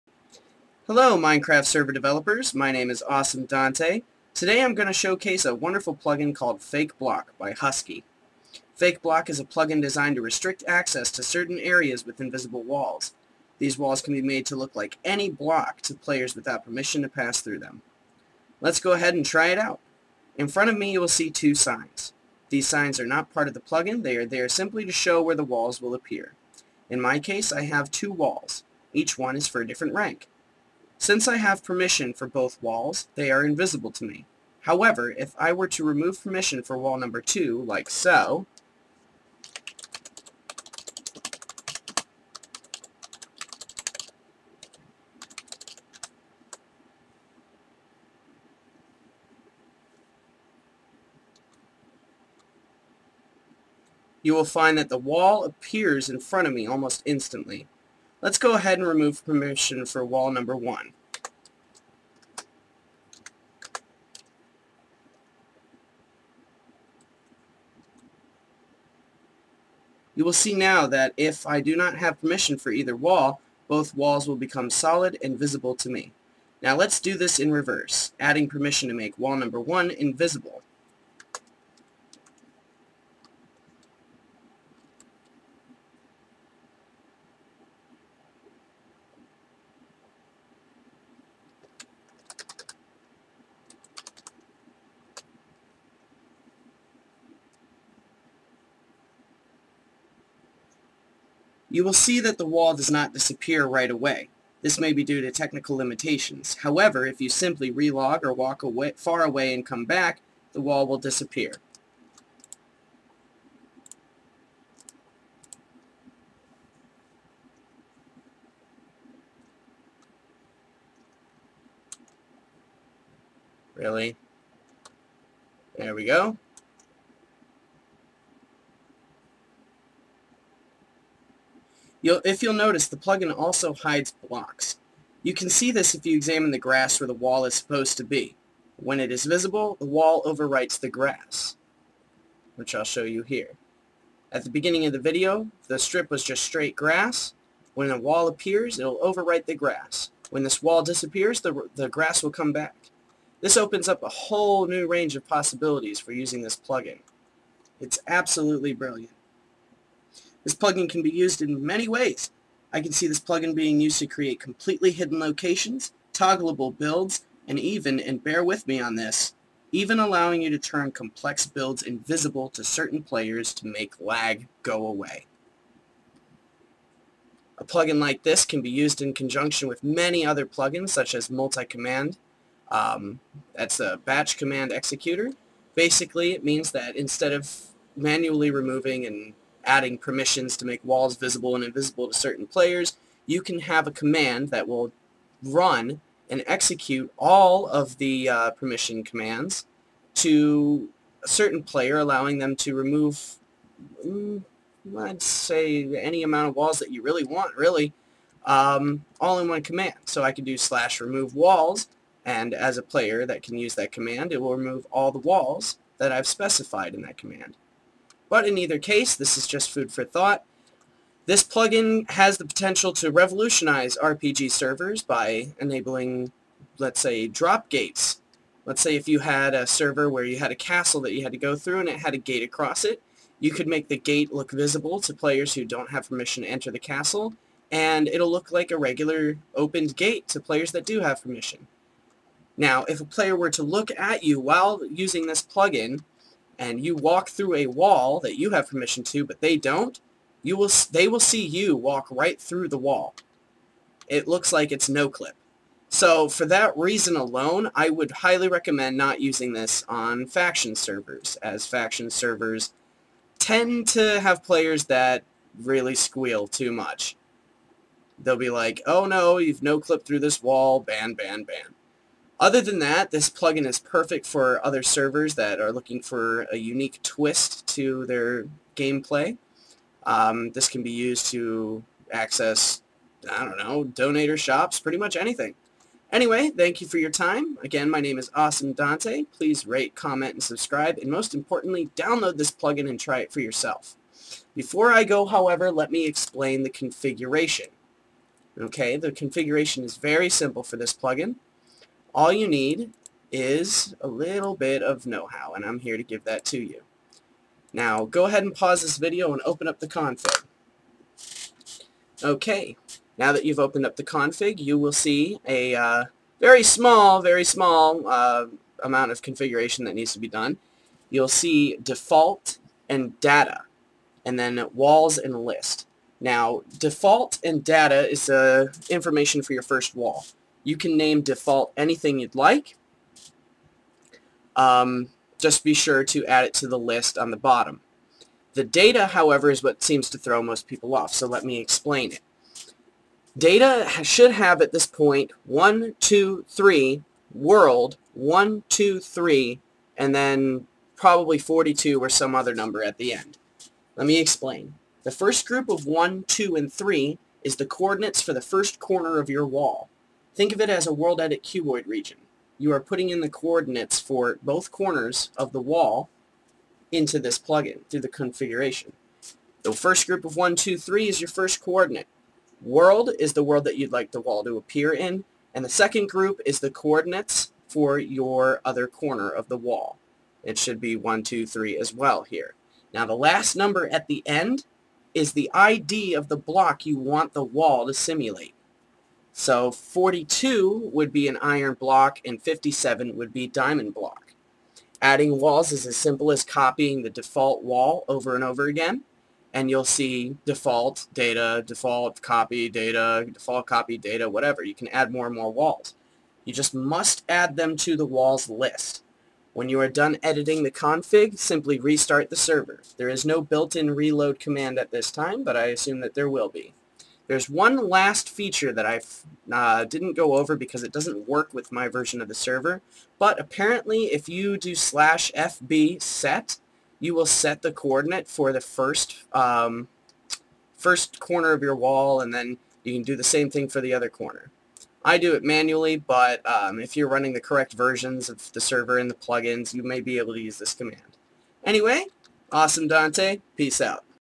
Spigot Plugin Showcase: FakeBlock by Husky
Expand description to see my config!

FakeBlock allows admins to create walls invisible to players with permission. Preventing those without permission from accessing the area guarded by the wall.

Also includes a little more info on how to configure the plugin